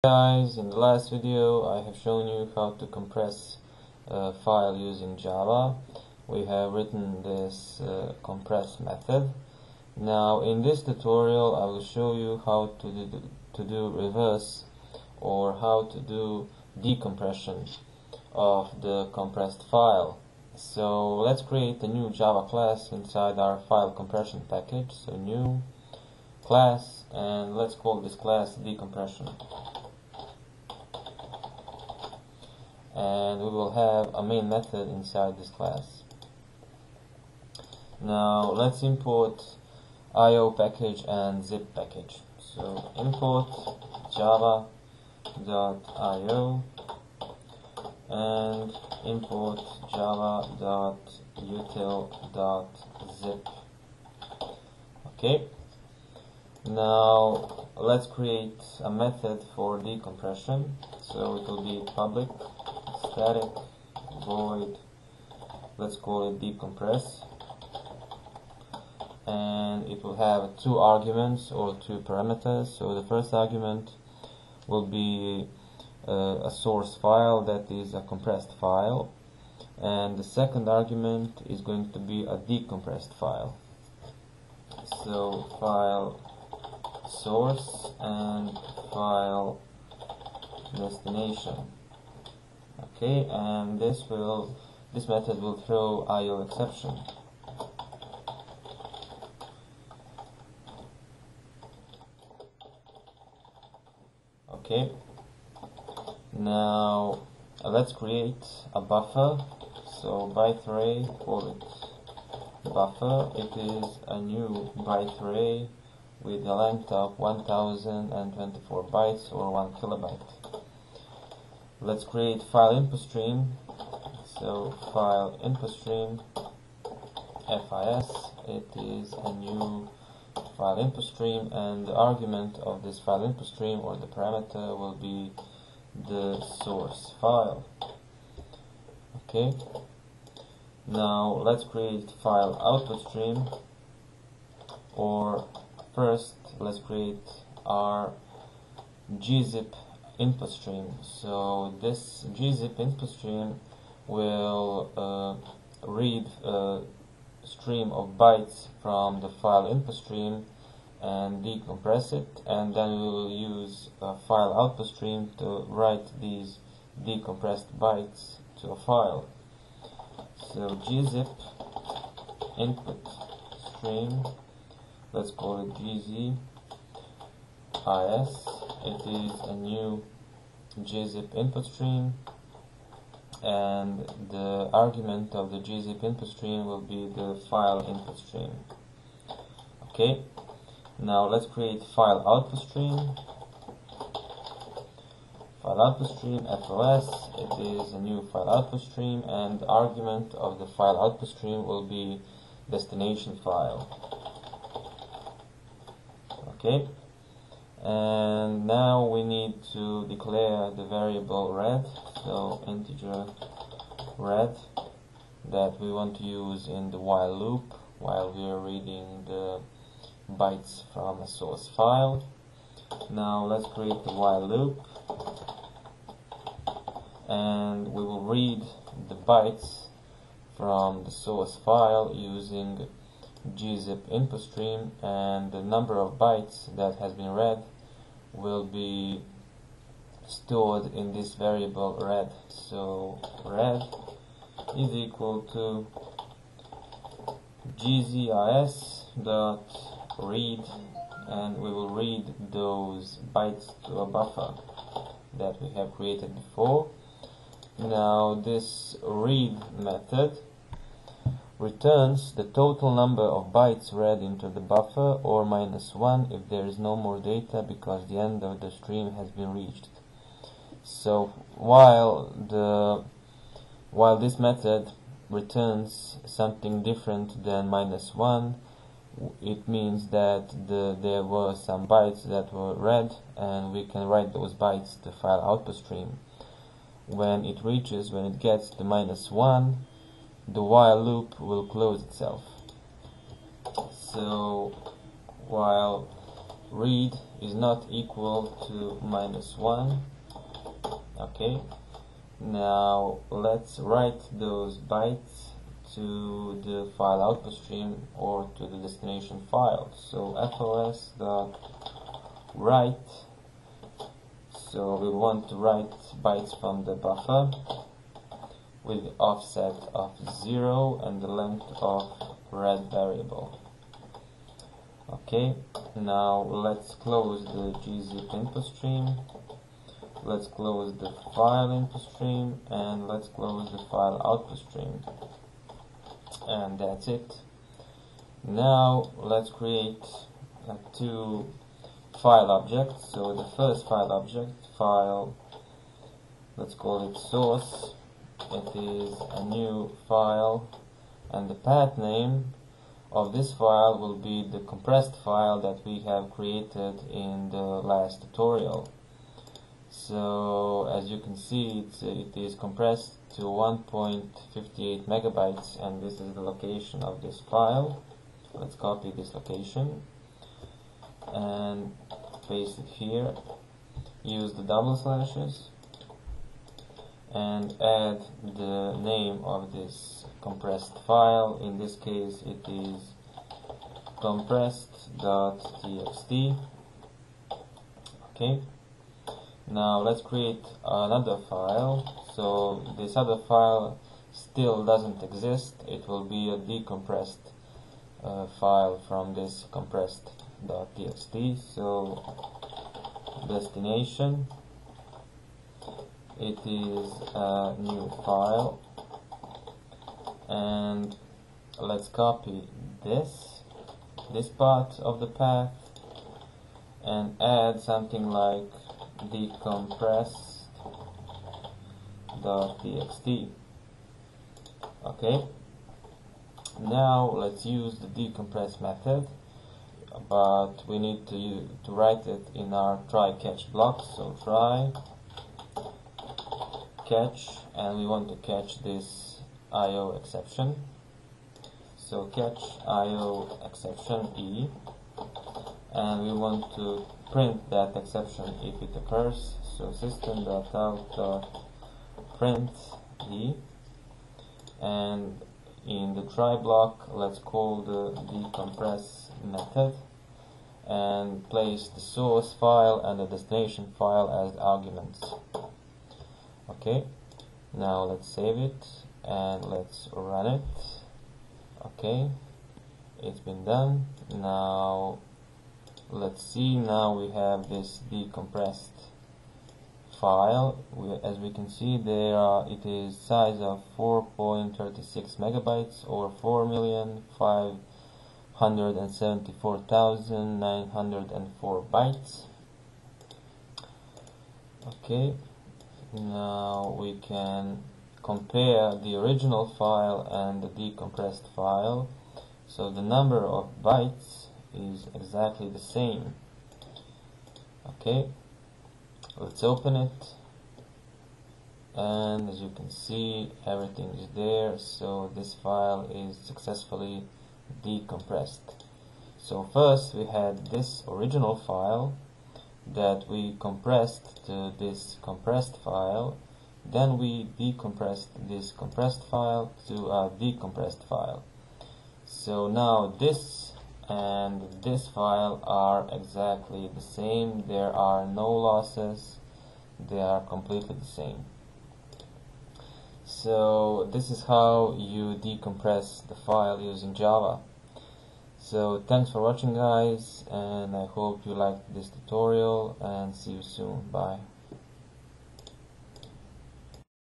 Java Tutorial - File Decompression
Video tutorial on how to decompress a file using Java